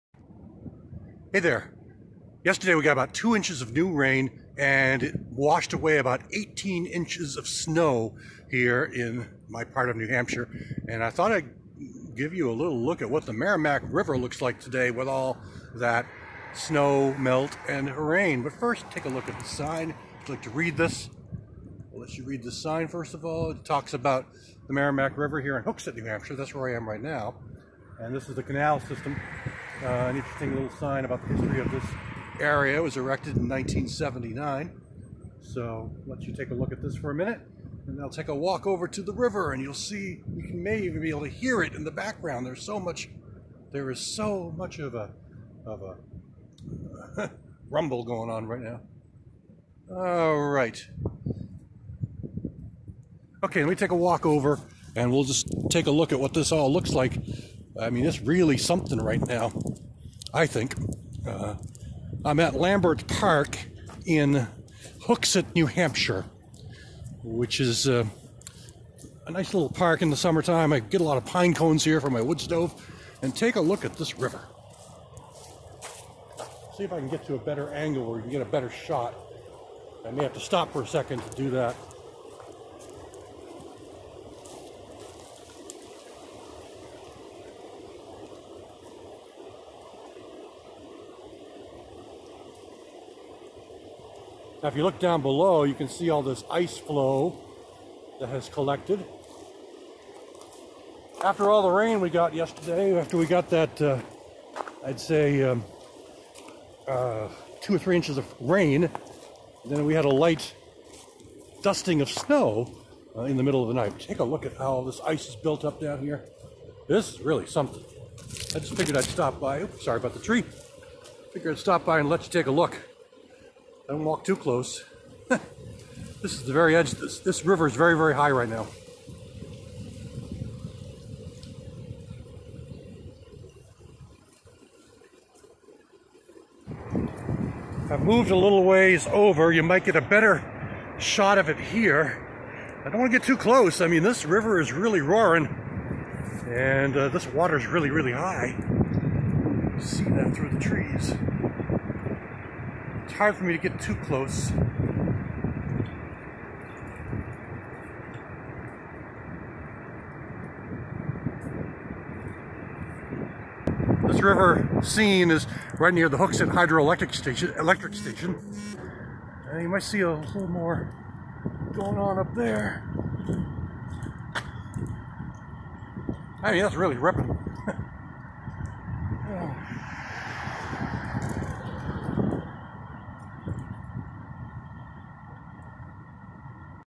High Water on the Merrimack River in New Hampshire.
A short video clip about the high water on the Merrimack River in Southern, New Hampshire after two inches of rain washed away 18 inches of previously fallen snow.